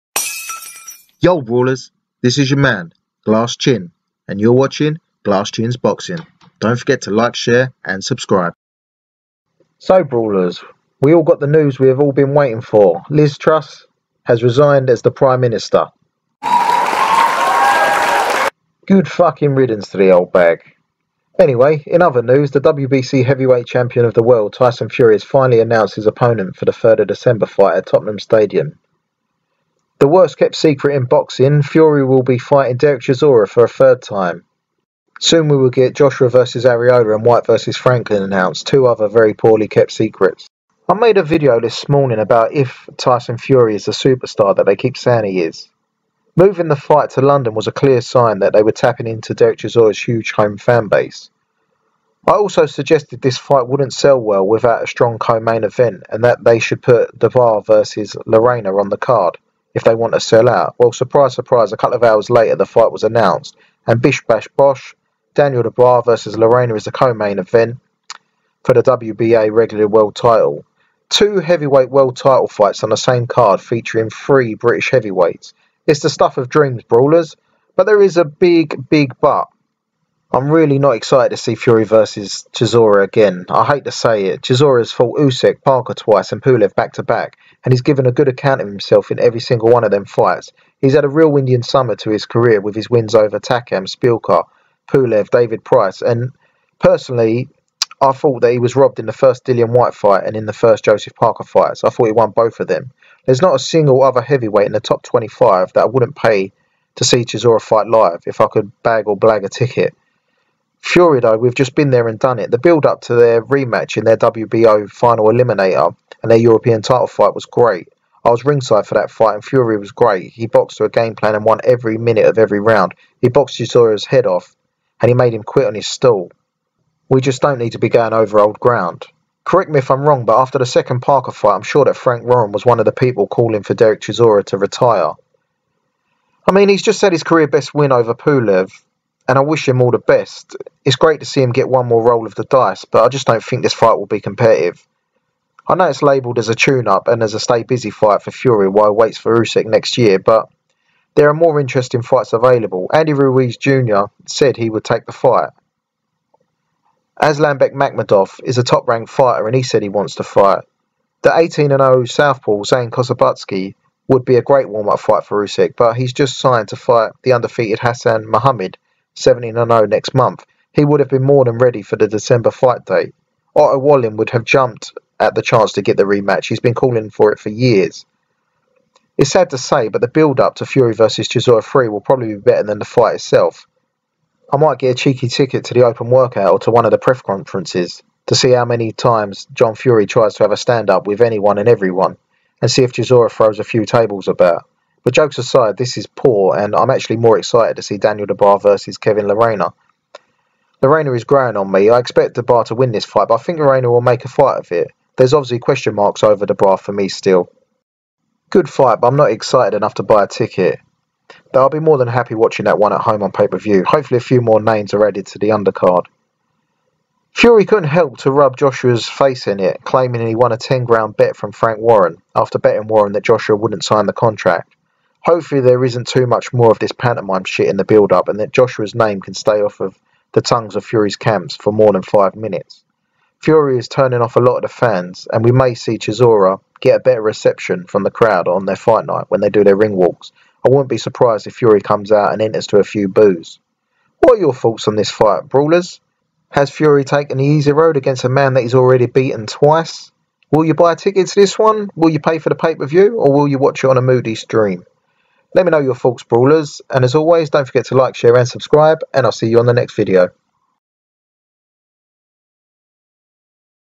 FURY vs CHISORA 3 announced!!!!!!!!!!!!!!
Tyson Fury vs Derek Chisora announced for the 3rd December at Tottenham Stadium for the WBC heavyweight title. Daniel Dubois to fight Kevin Lerena for the WBA regular title on the co main event. Fury bet Frank Warren 10k that Joshua wouldn't sign the contract.